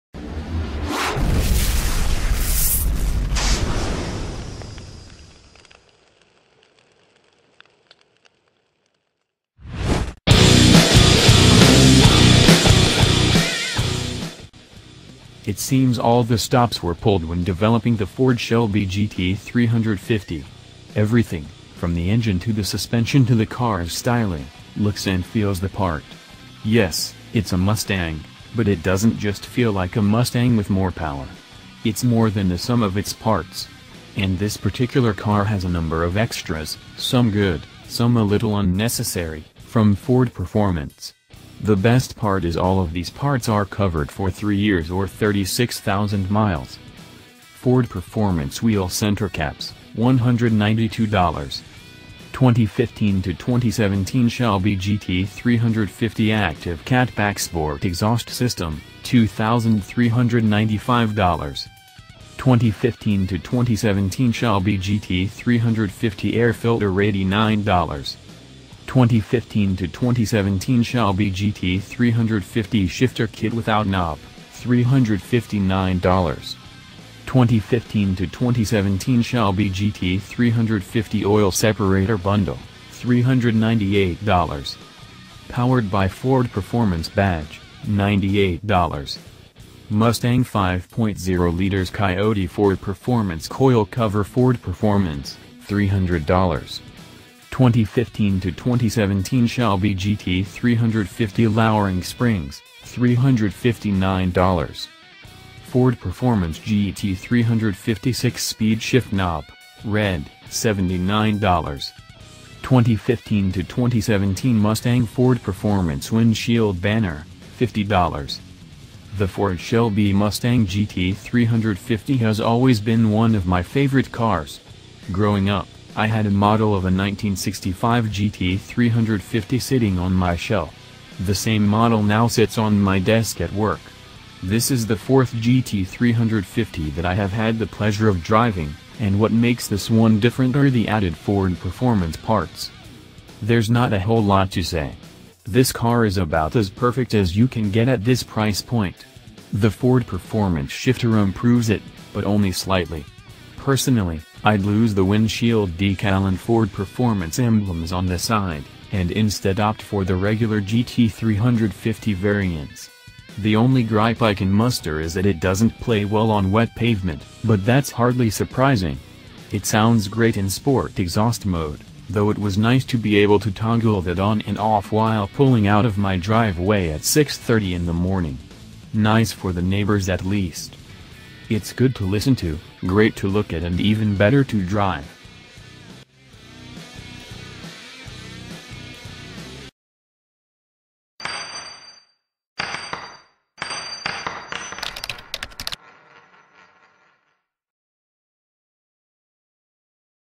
HOT !!! 2017 FORD SHELBY GT 350 FULL REVIEW
HOT !!! 2017 FORD SHELBY GT 350 FULL REVIEW - It seems all the stops were pulled when developing the Ford Shelby GT350. Everything, from the engine to the suspension to the car's styling, looks and feels the part. Yes, it's a Mustang, but it doesn't just feel like a Mustang with more power. It's more than the sum of its parts. And this particular car has a number of extras (some good, some a little unnecessary) from Ford Performance. The best part is all of these parts are covered for 3 years or 36,000 miles.

Ford Performance wheel center caps - $192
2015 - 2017 Shelby GT350 active cat back sport exhaust system - $2,395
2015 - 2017 Shelby GT350 air filter- $89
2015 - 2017 Shelby GT350 shifter kit without knob - $359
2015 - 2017 Shelby GT350 oil separator bundle - $398
Powered by Ford Performance badge - $98
Mustang 5.0L Coyote Ford Performance coil cover Ford Performance - $300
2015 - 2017 Shelby GT350 lowering springs - $359
Ford Performance GT350 6-speed shift knob - red - $79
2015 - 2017 Mustang Ford Performance windshield banner - $50